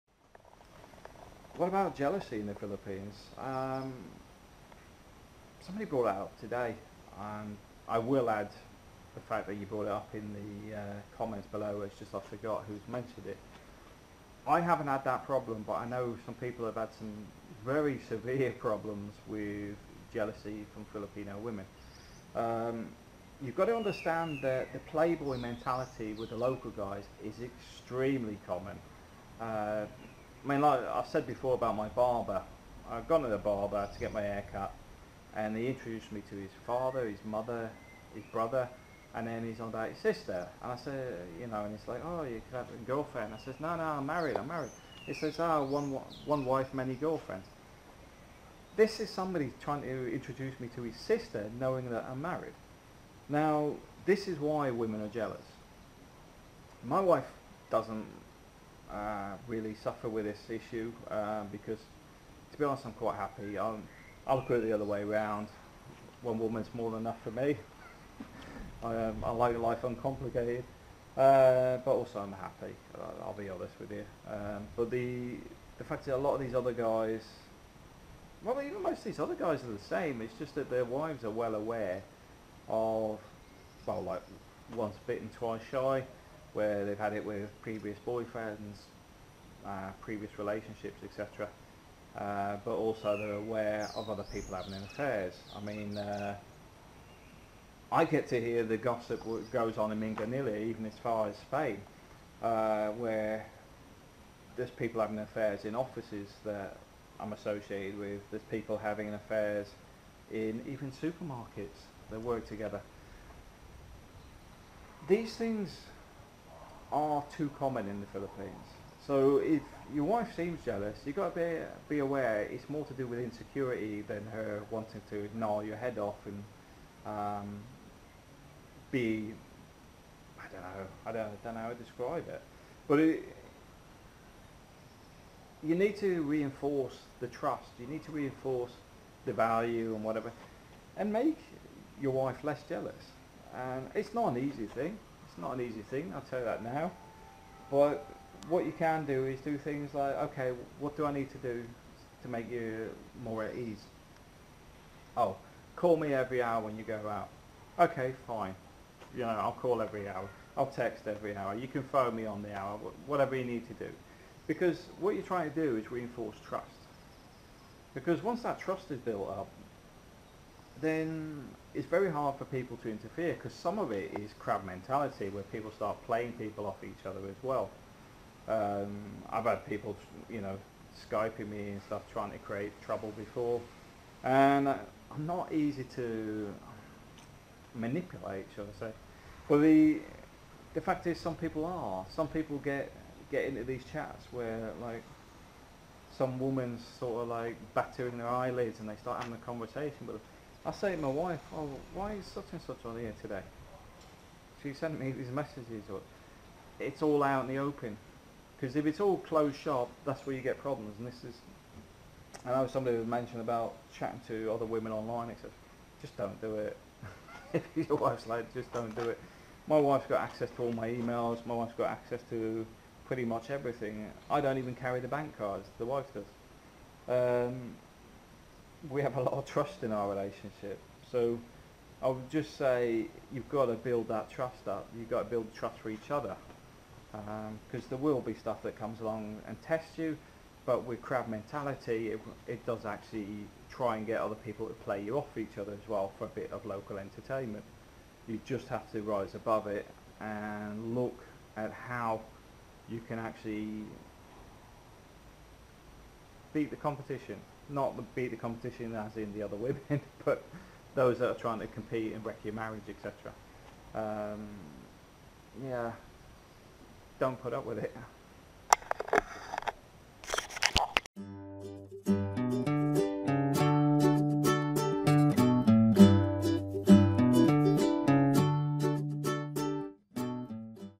Philippines Expat Jealous Filipinas
Jealousy in Filipina's can be a risky business but also can destroy a relationship. I have seen it literally do it to other people, girlfriends and wives becoming stroppy and argumentative. Threatening to leave the guy etc. but in all cases the guy had literally done nothing to cause the issue. Well not in the way that you would expect for example a guy wanting a bit of space. In the UK its very common to go walking alone to gather thoughts or to get peace and quiet. But in the Philippines going somewhere alone is unusual and not seen as normal. As such you become questionable on why your "sneaking out" when your actually just going for a quiet walk.

It all comes down to insecurity, in the Philippines women approach you regularly but I am more in the mindset of letting people be people. If someone is going to leave me then they would do at some point anyway. So why make everyone's life a misery based on something that hasn't happened (at least yet).

My best advice is to reinforce security though as over time things should become easier unless you end up with the nightmare partner who already assumes your having an affair regardless of what you are doing or have done. In that case best advice is leave!